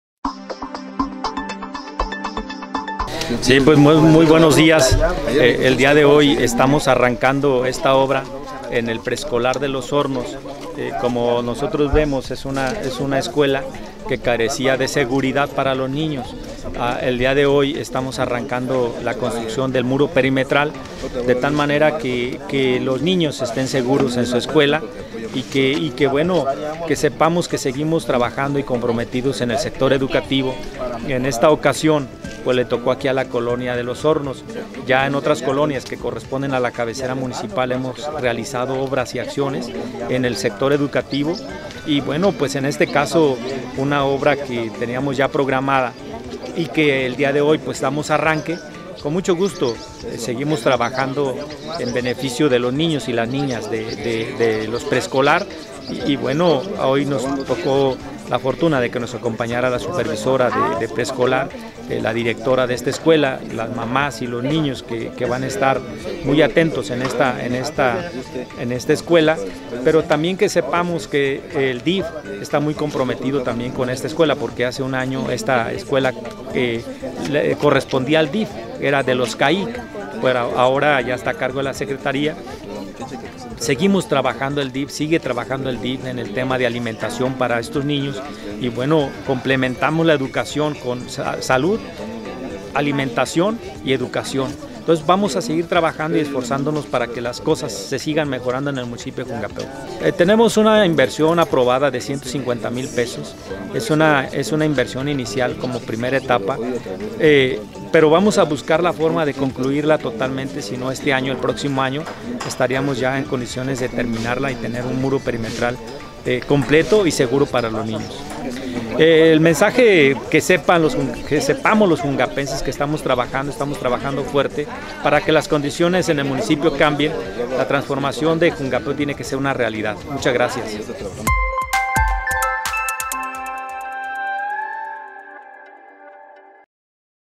José Lugo Rodríguez da banderazo a obra en Jungapeo
El presidente Municipal de Jungapeo el Arq. José Lugo Rodríguez, dio bandarazo de inicio de la construcción de un cerco perimetral, en el Preescolar de la localidad de Los Hornos.